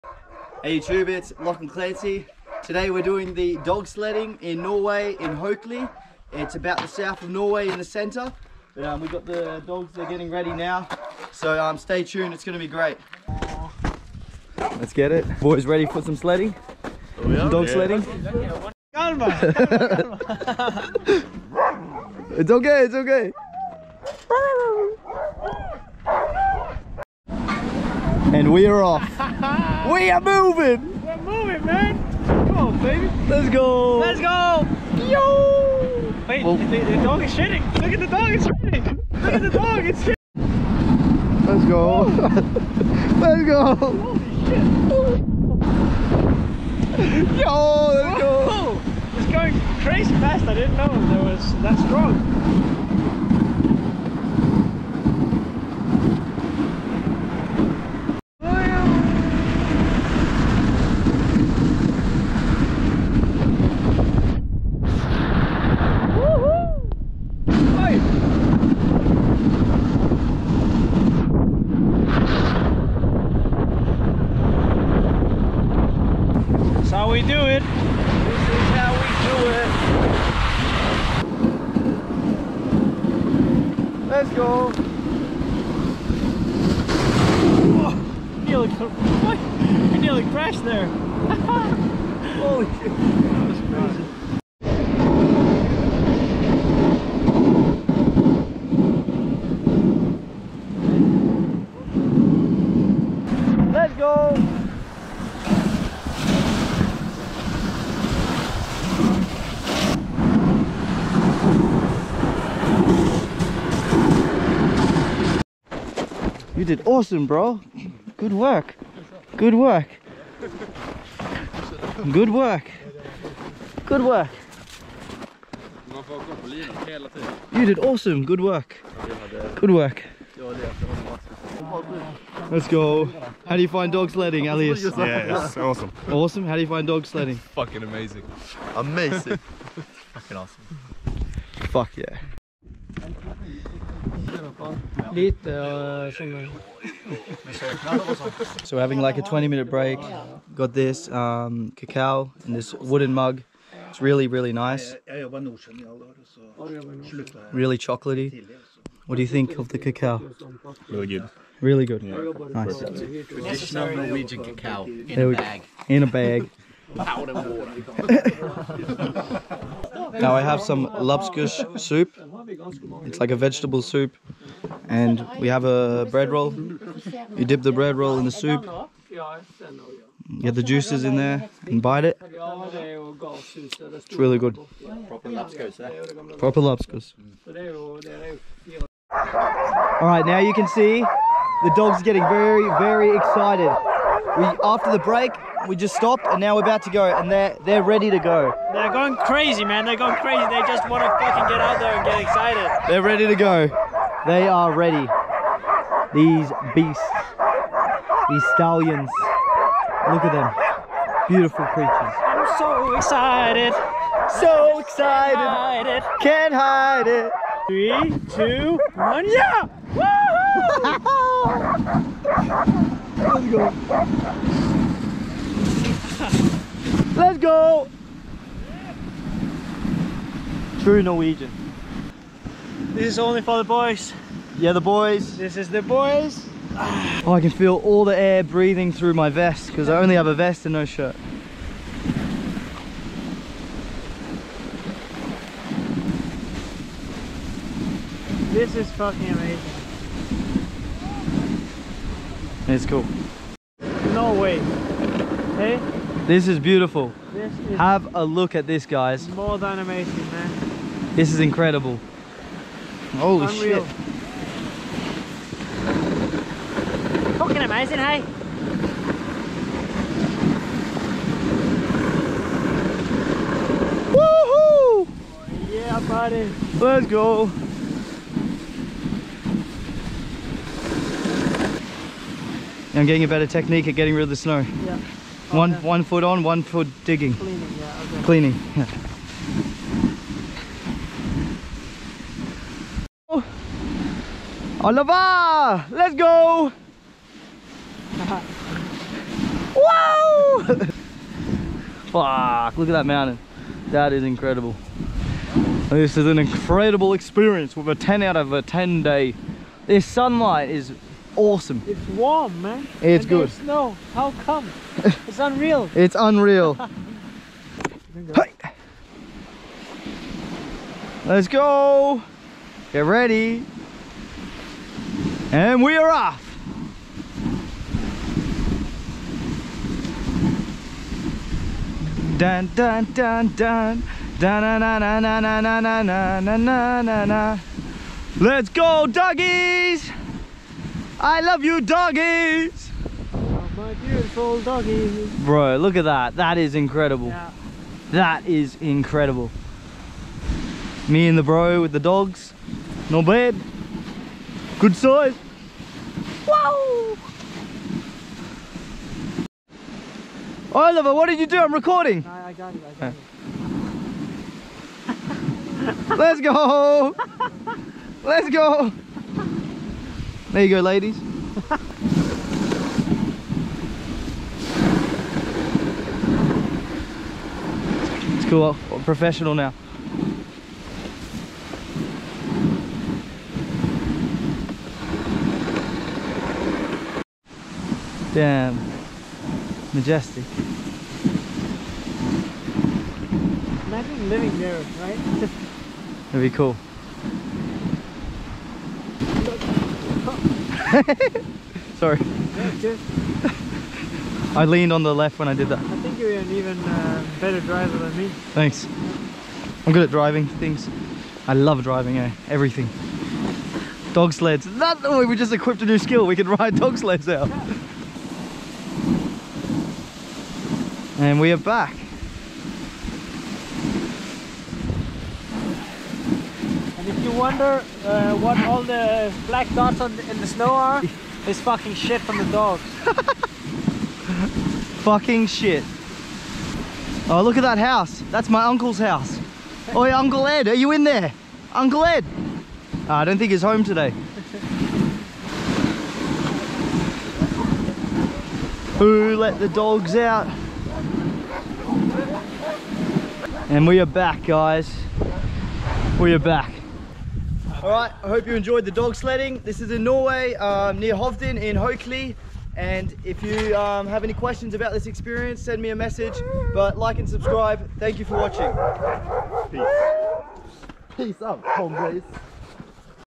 Scenic Dog Sledding in Norway!
In this video you will see my friends and myself dog sledding at Haukeli Husky which is located in the south of Norway. I found this experience really enjoyable and the vegetable soup was fantastic half way through the ride. All the dogs were extremely cute and it was amazing to be around 40 dogs at once. The energy was unreal. Yes this is a little pricey $$, but it's totally worth it!